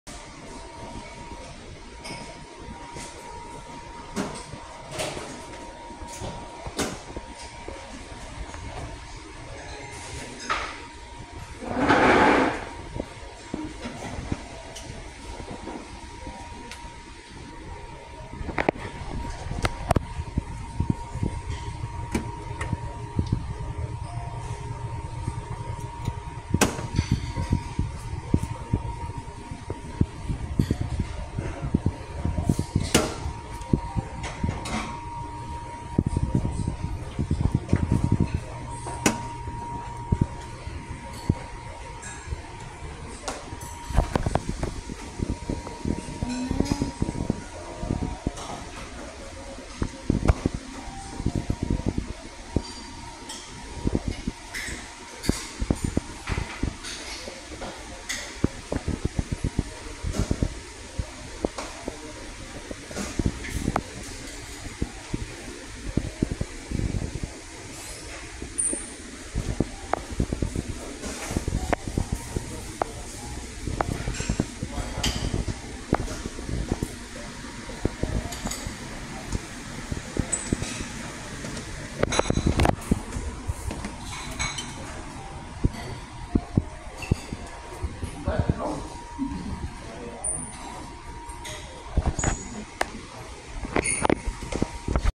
Emirates Marhaba Lounge at NAIA Terminal 3
Marhaba Lounge is located at the 4th level of Ninoy Aquino International Airport Terminal 3. Emirates Business Class passengers have access to this lounge.